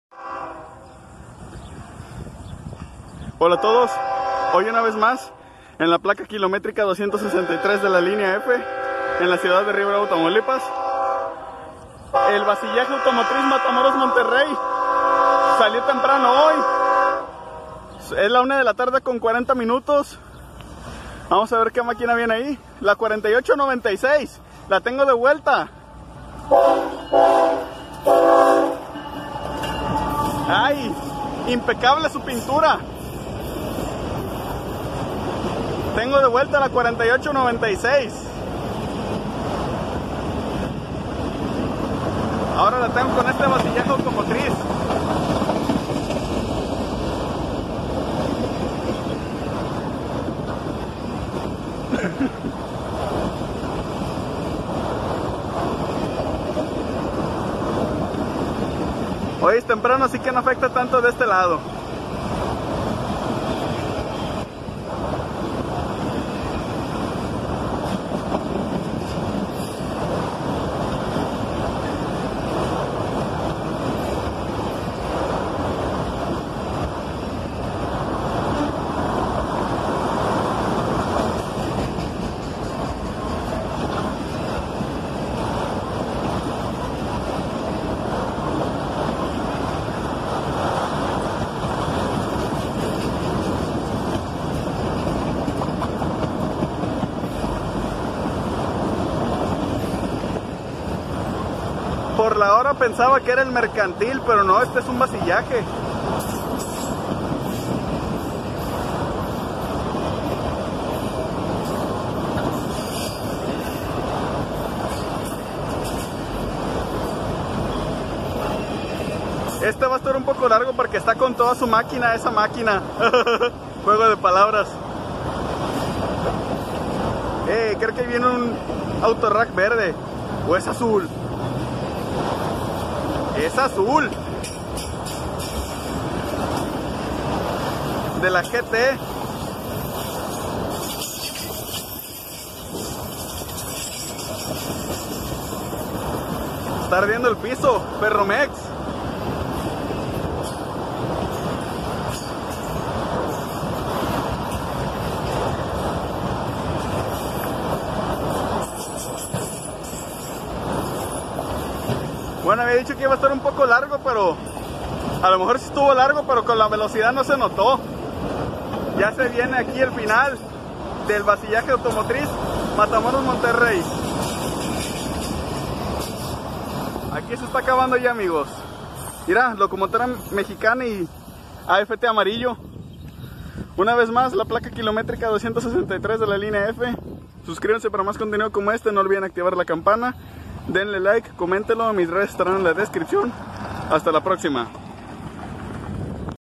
El Vacillaje A-MTMY con la KCSM 4896 ES44AC sola. Regreso épico al canal.
Hola a todos. Hoy les traigo un nuevo tren. Una vez más el Vacillaje Automotriz Matamoros-Monterrey pasando por la placa kilométrica 263 de la línea F en la ciudad de Río Bravo, Tamaulipas, y siendo estirado por una ES44AC de Kansas City Southern de México con el número 4896.
Después de poco más de dos meses, la vuelvo a grabar, ahora estirando un tren con AFT americano. Con esta máquina inició la actividad masiva en mi canal. 😎
Ahí publico sobre mis próximas subidas.
Este video es apto para todo público (COPPA).